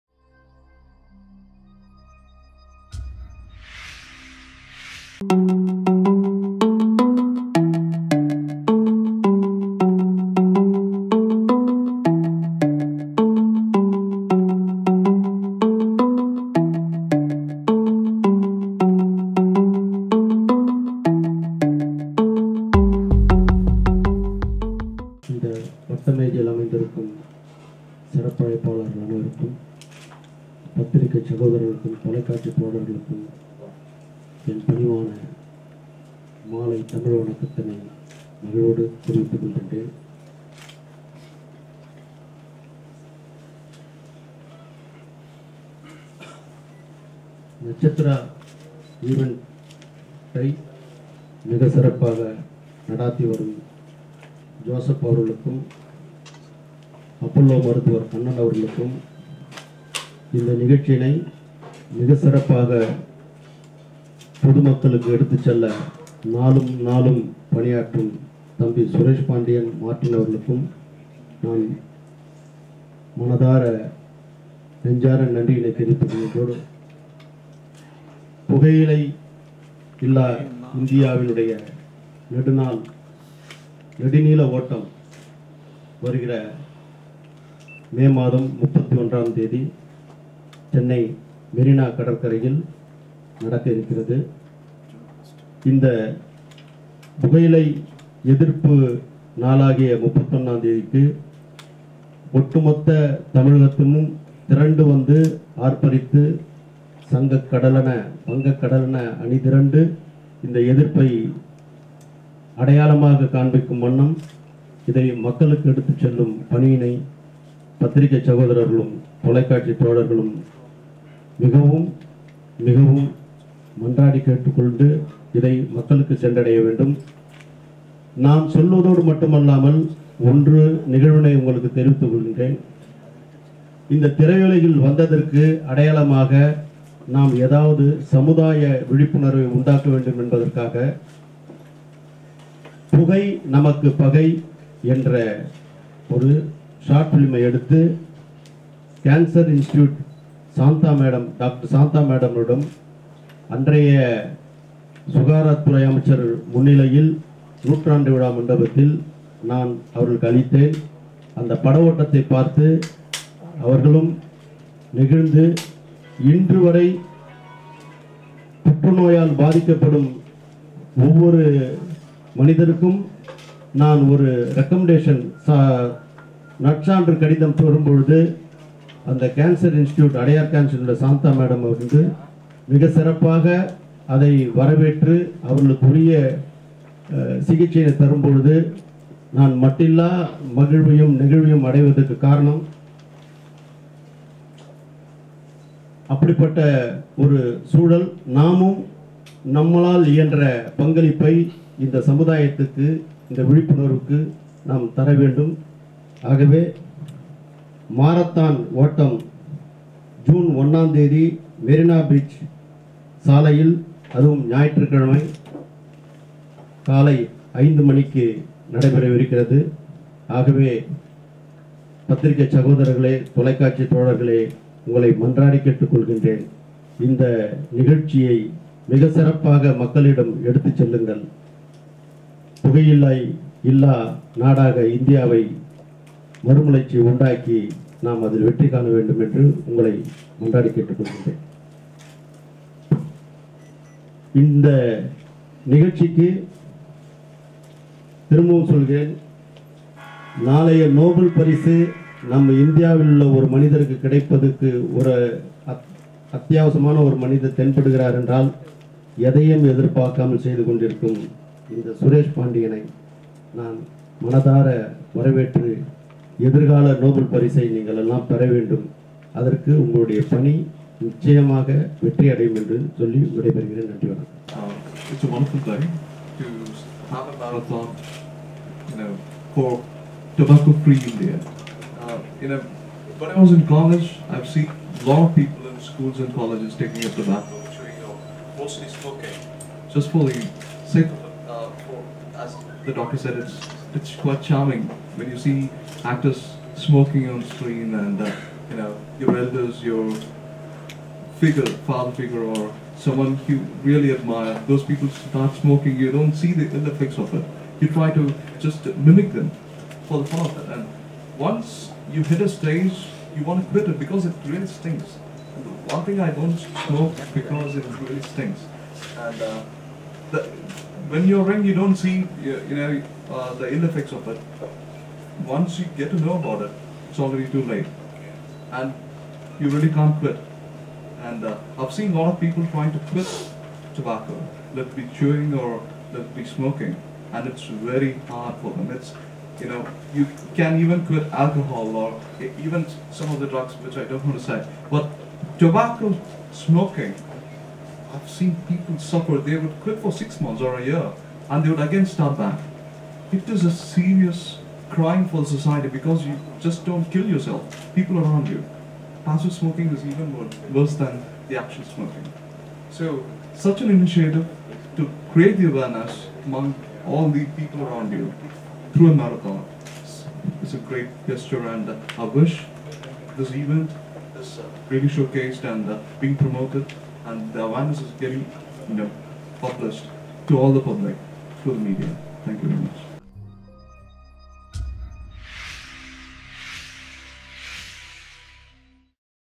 Tobacco Free India Press Meet
Tobacco Free India Press Meet held on 23rd May 2014 at Tharamani, Chennai. Thaman Kumar, Kalaipuli S. Thanu, Kala Bala Sundaram graced the event.